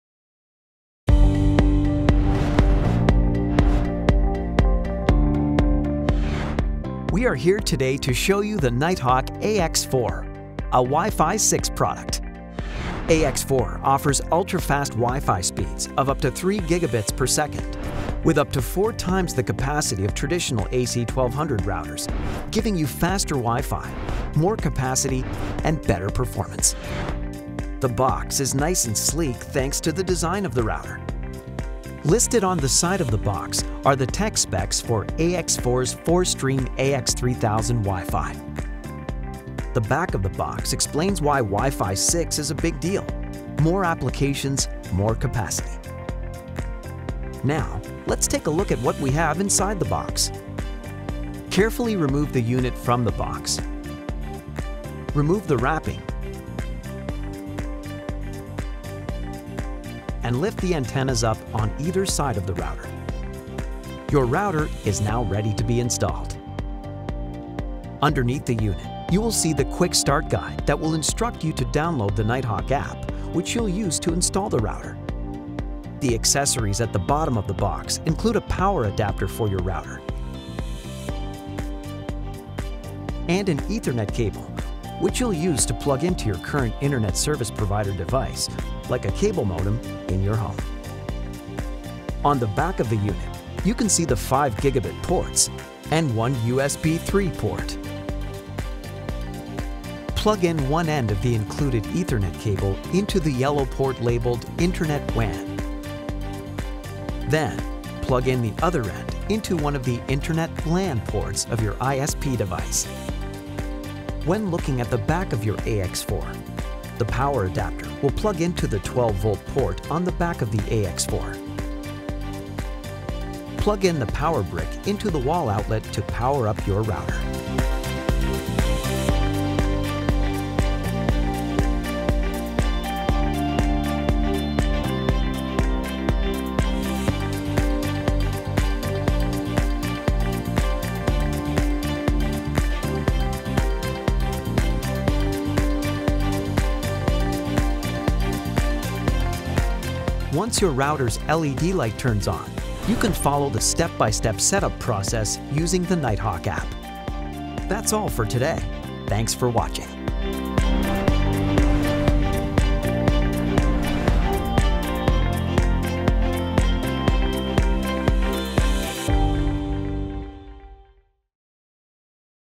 Unboxing the NETGEAR Nighthawk AX4 4-Stream WiFi 6 Router | RAX40
Learn more about the NETGEAR Nighthawk AX4 AX3000 4-Stream WiFi 6 Router

The Nighthawk AX4 (RAX40) joins NETGEAR's lineup of WiFi 6 routers. Check out what's inside the box and see how easy it is to set up using the Nighthawk App. Get ready to experience the next generation of WiFi with the NETGEAR Nighthawk AX4 4-Stream WiFi 6 Router.

Built with the latest generation WiFi 6 technology, get 4 times more network capacity compared to today's WiFi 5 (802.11ac) routers. Experience a new level of gigabit connectivity so you can connect, stream, and download faster to your all devices.

Set up your NETGEAR Nighthawk AX4 WiFi 6 Router by simply plugging into your cable modem, and downloading the Nighthawk App. Your home network has the improved capacity  to handle the devices of today and tomorrow.